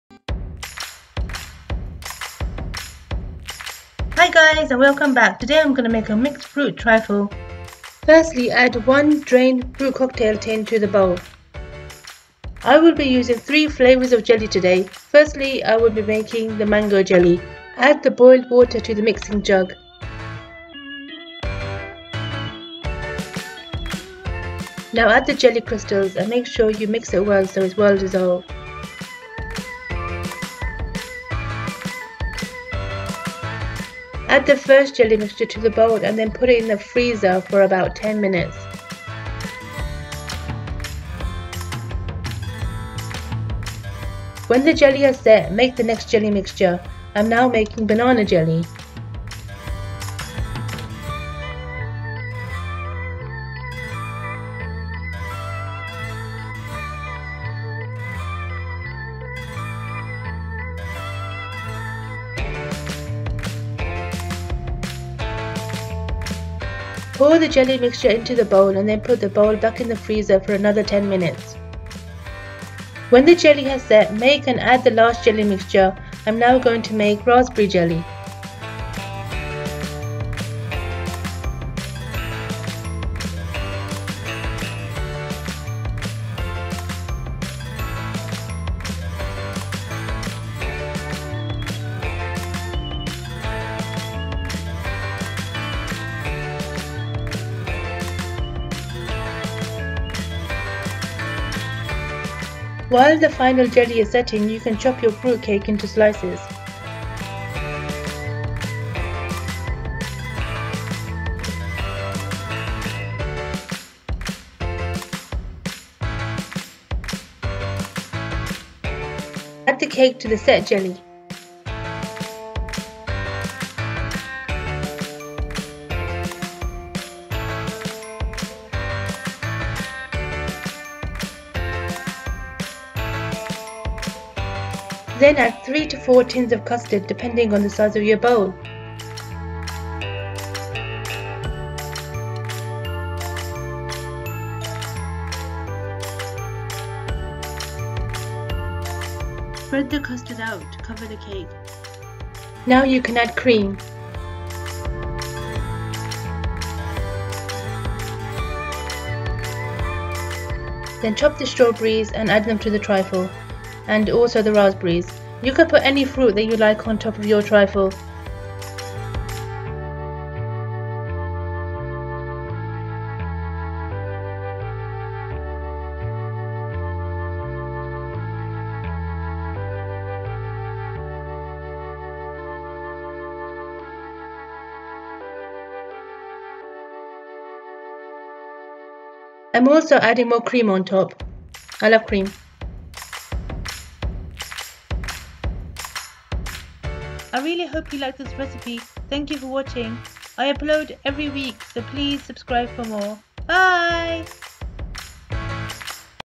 Quick and Easy | Mixed Fruit Trifle
Hi Guys

In this video i show how i make my Mixed Fruit Trifle, i hope you like it, please leave suggestions in the comments of what you would like to see me make next.

Ingredients:

Custard tins 3-4 depending on the size of your bowl
Fruit Cake 1 or 2 depending on size of your bowl
Jelly 3 different flavours
Fruit cocktail tin 1 - Drained
Whipped Cream
Strawberries
Raspberries
(You can use any of your favorite fruits to decorate the trifle with)